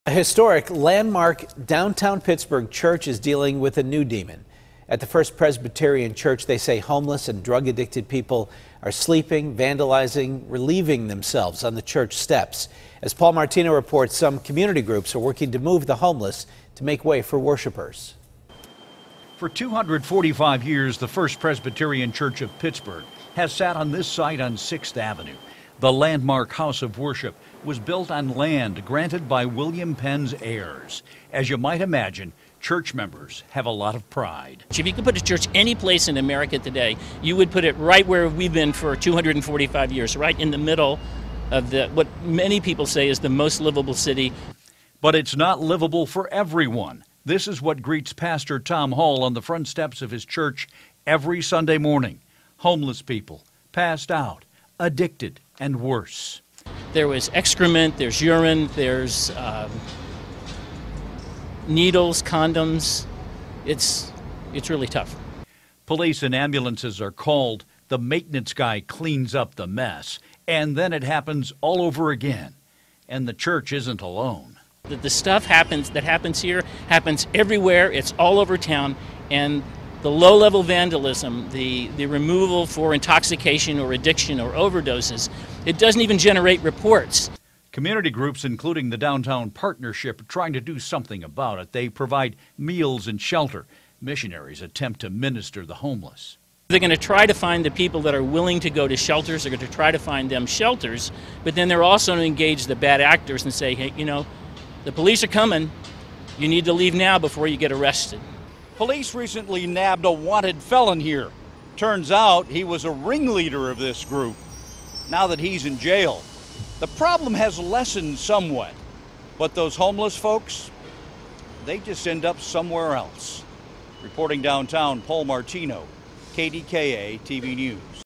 Historic Church Dealing With Vandals, Other Issues
A historic, landmark church downtown is dealing with a new demon - drug addiction.